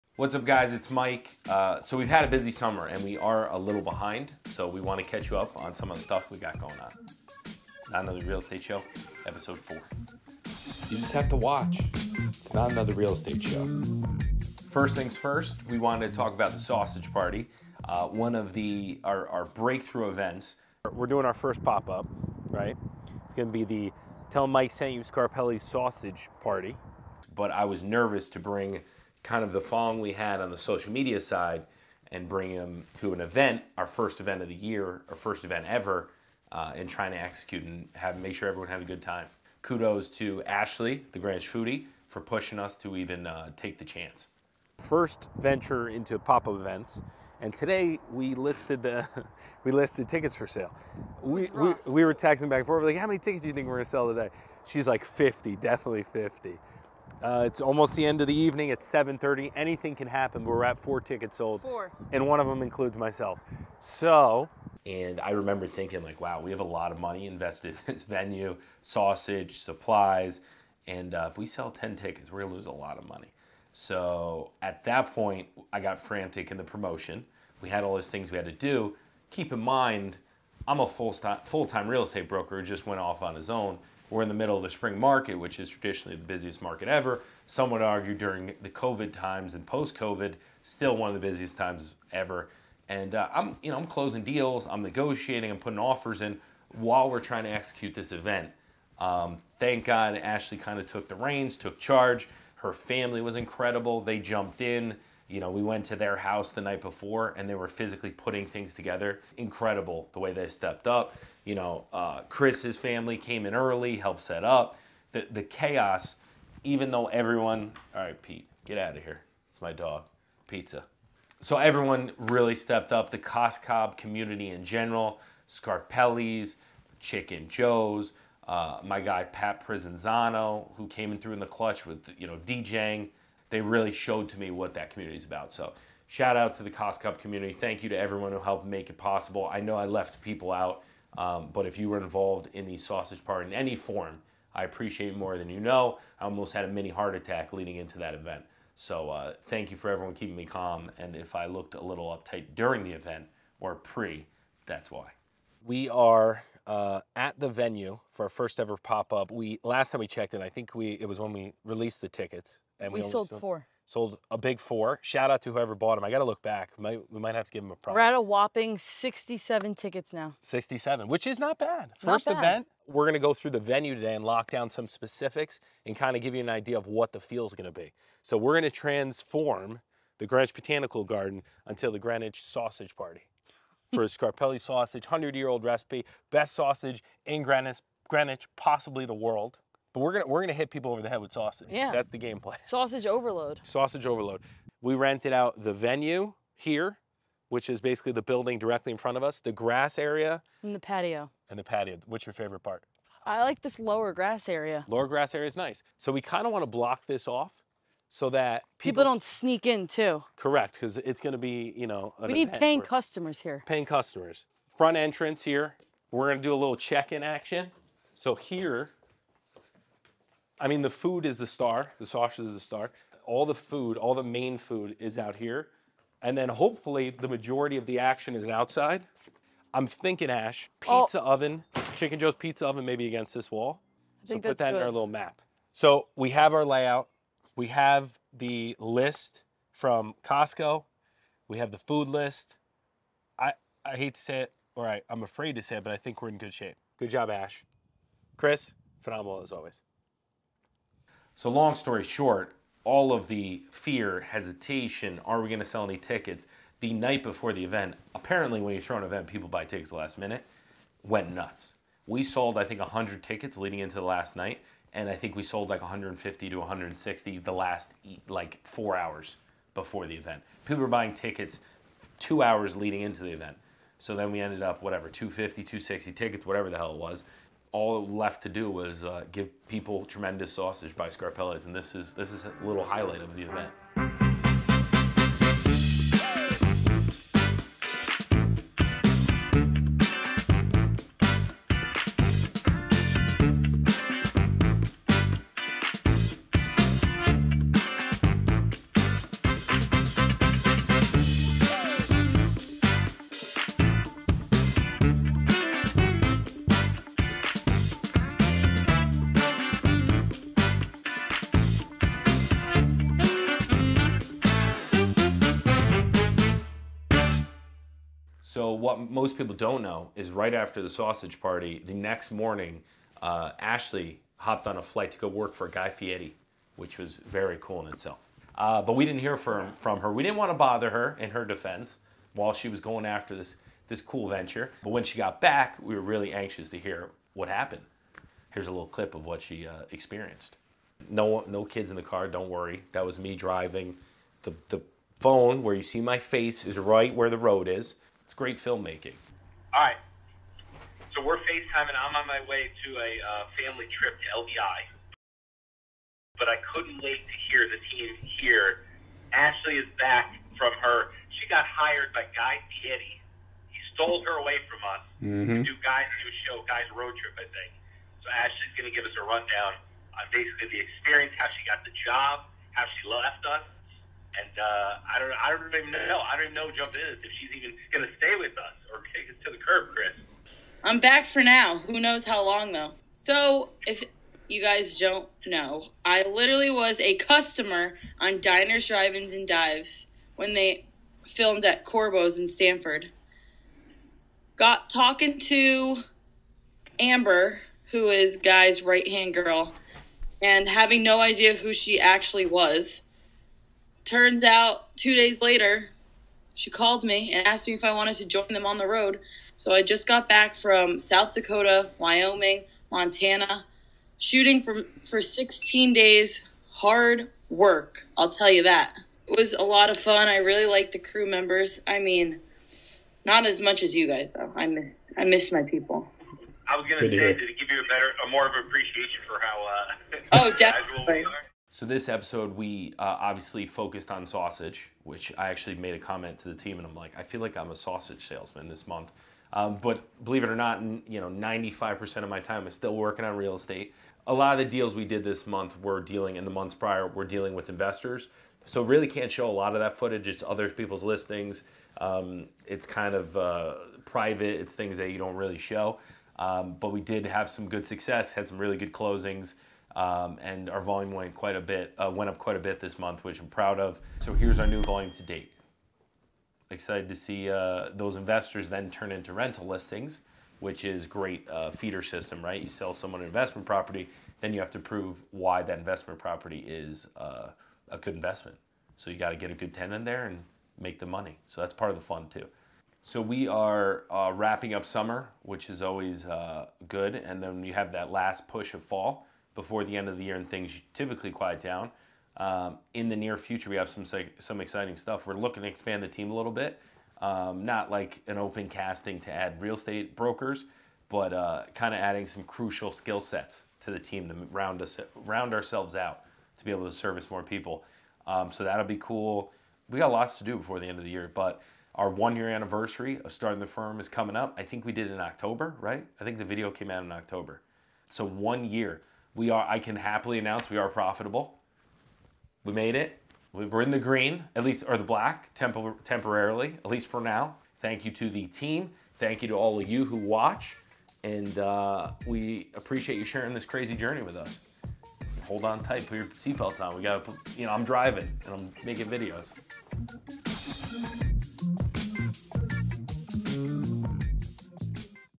Episode 4 - Building a Real Estate Empire in Greenwich CT | Mike Parelli Greenwich Real Estate
In our new docu-series we're documenting our journey of building our real estate brokerage and portfolio in one of the most competitive and expensive real estate markets in the world. We're starting from scratch from a home office and a vision to become a name synonymous with Greenwich Real Estate. It’s more than just a real estate show, it’s notanotherrealestateshow ... Just watch.

Follow the NARES Team;